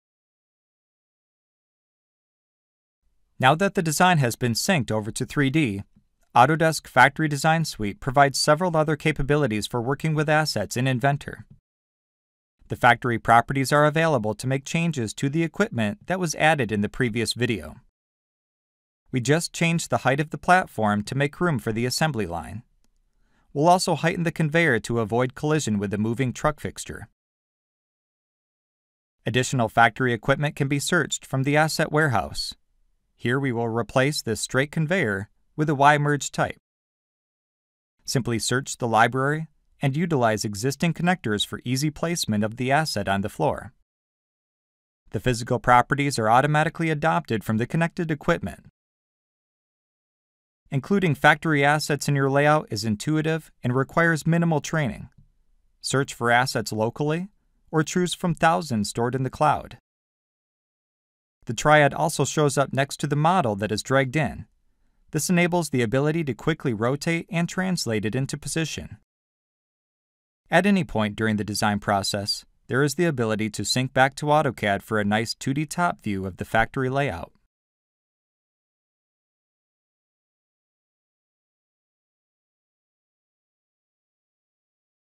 Factory Layout Design - Part 2 of 4 (Inventor)
Using Autodesk Factory Design Suite to design and optimize your factory layout.